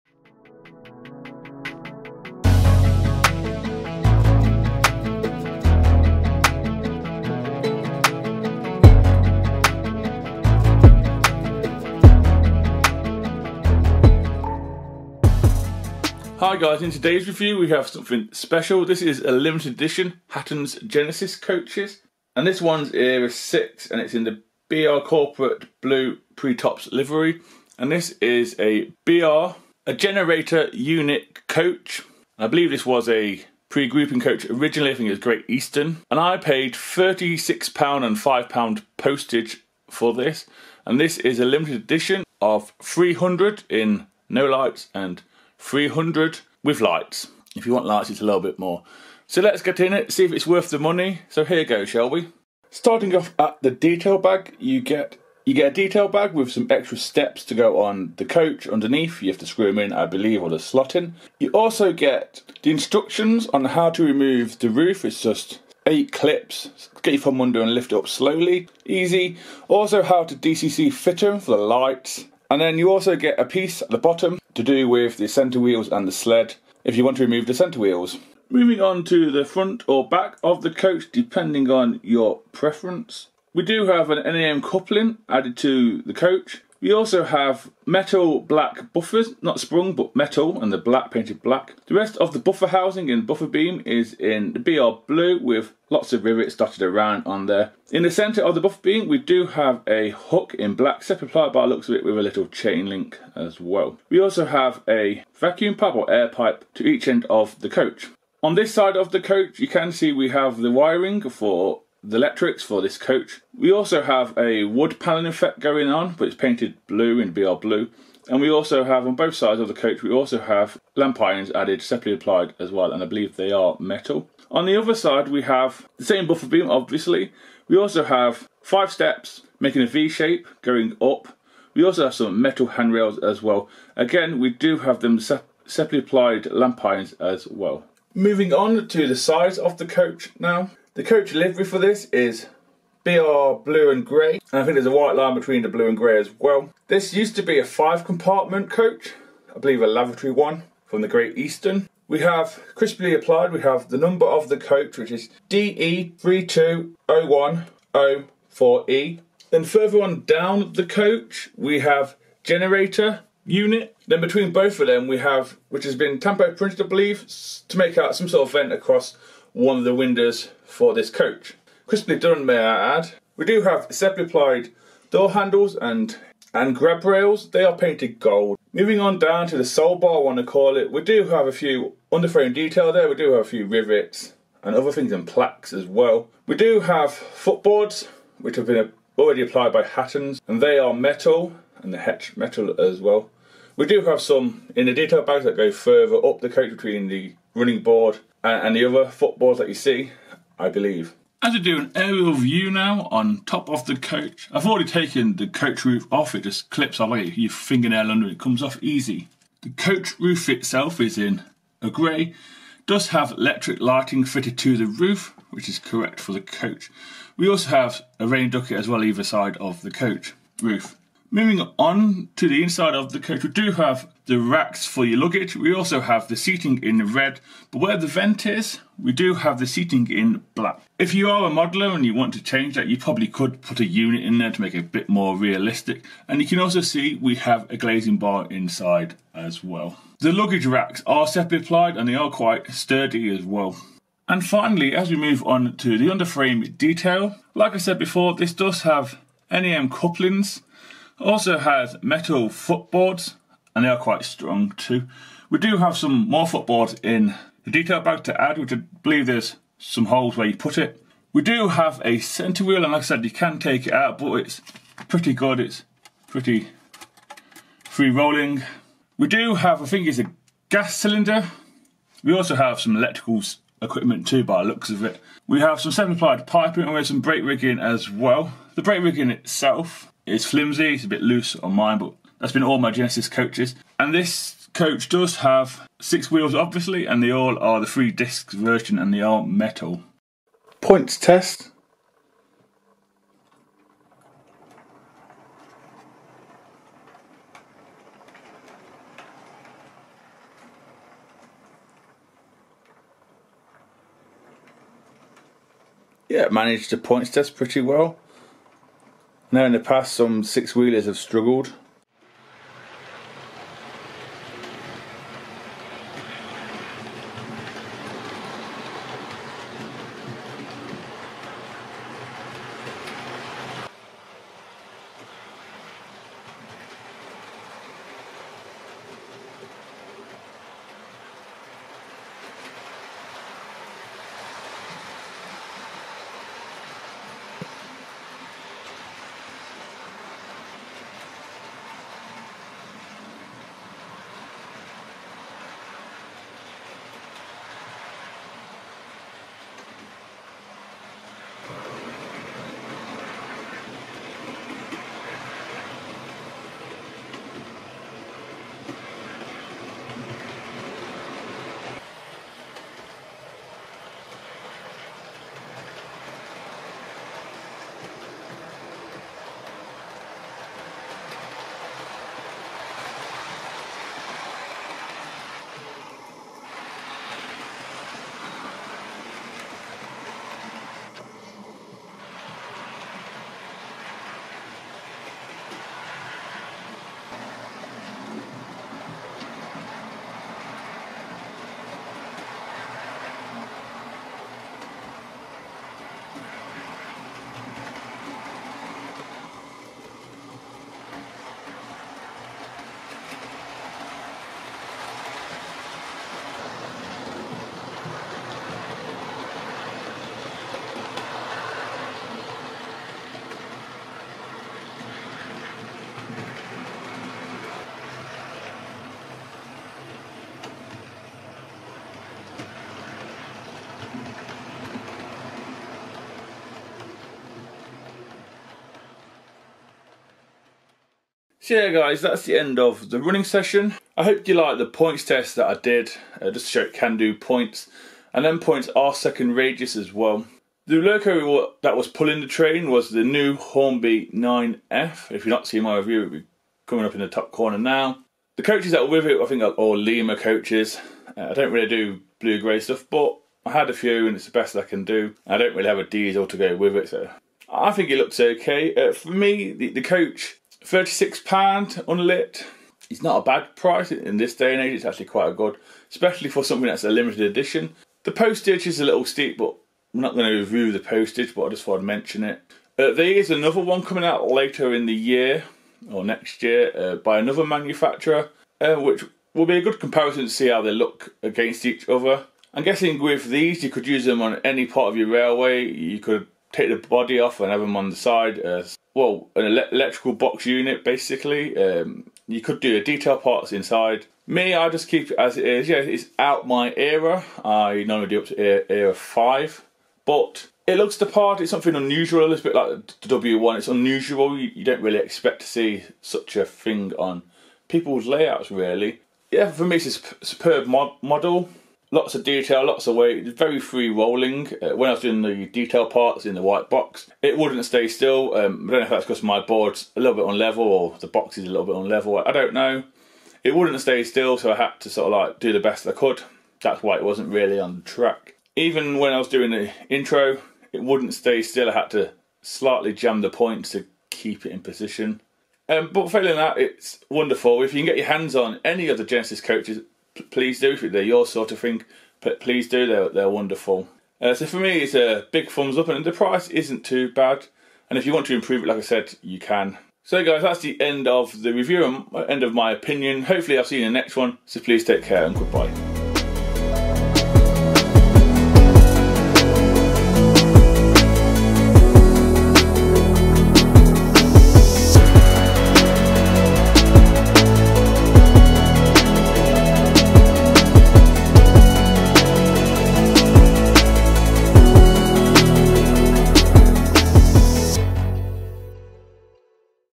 LIMITED EDITION: REVIEW HATTONS BR GENERATOR UNIT COACH
HI GUYS AND GIRLS
HERE MY REVIEW ON A LIMITED EDITION MODEL FROM HATTONS GENESIS COACH IN BR LIVERY AND IT GENERATOR UNIT COACH
HOPE YOU LIKE THE REVIEW
KEEP SAFE ARP